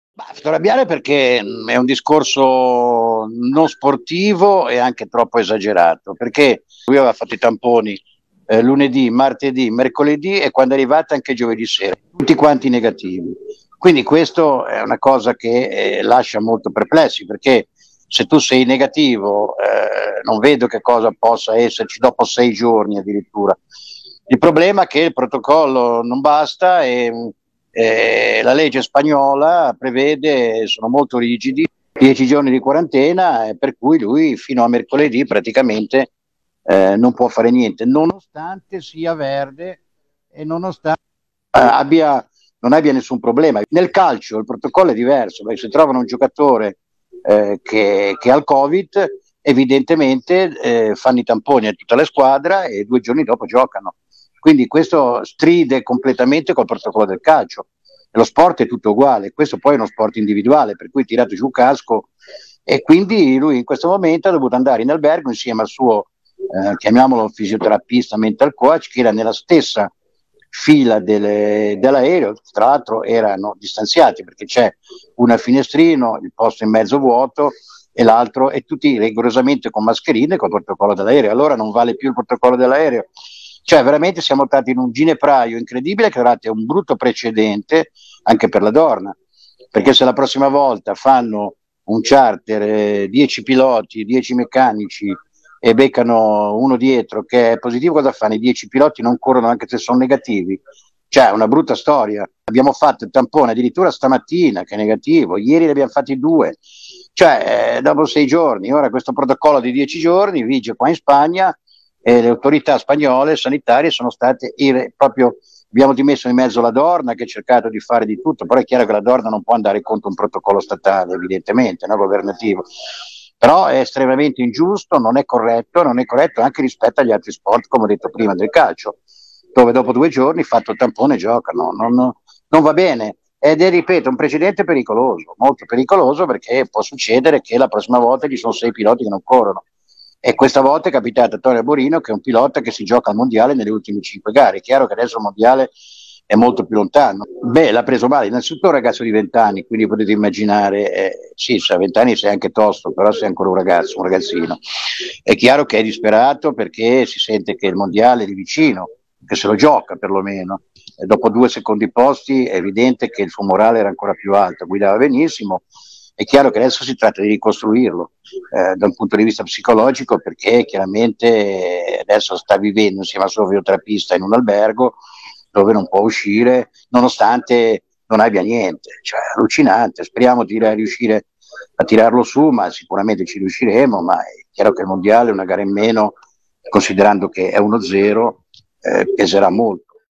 Caso Arbolino, Pernat non ci sta
Moto3, sull'esclusione di Arbolino parla il manager: "Discorso non sportivo e troppo esagerato"